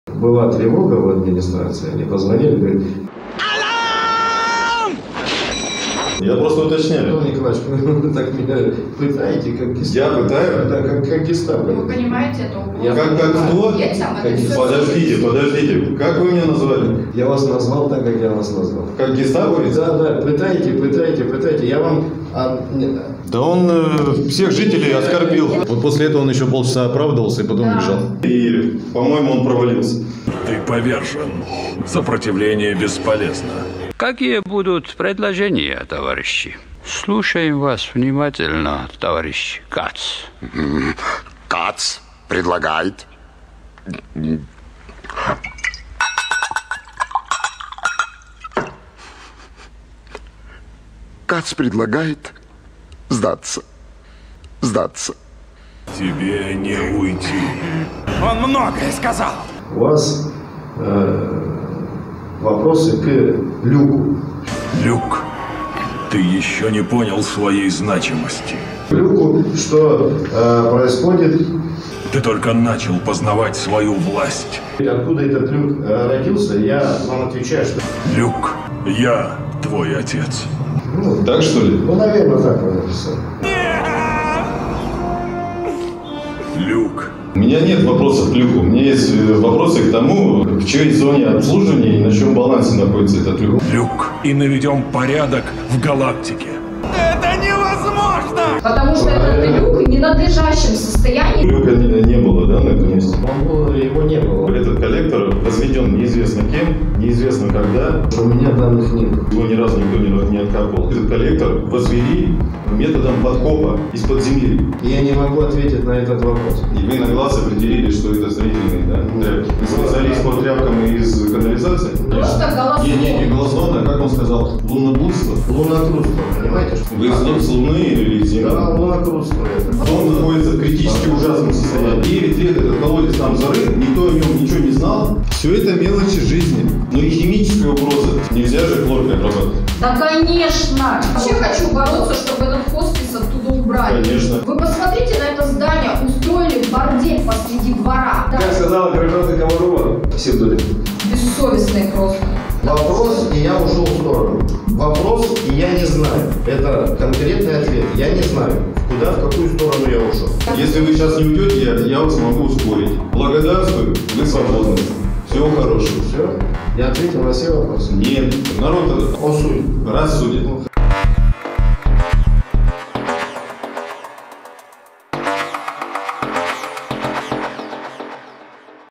Трейлер ЗАБОТЛИВОЕ СЕРДЦЕ или ПЫТАЕТЕ КАК ГЕСТАПОВЕЦ 2023.07.24 Сургут
новые подробности про шесть разливов канализации посередине детской площадки из коллектора, который был как минимум 5 лет закопан под песком и никем не обслуживался и в который выкидывали памперсы и медицинские отходы из организации АНО "Заботливое сердце", а так же откровенное "лунокрутство" заместителя директора ООО "УК ДЕЗ ВЖР" Черников Андрей Александрович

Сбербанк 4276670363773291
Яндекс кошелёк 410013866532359

на ты или на вы?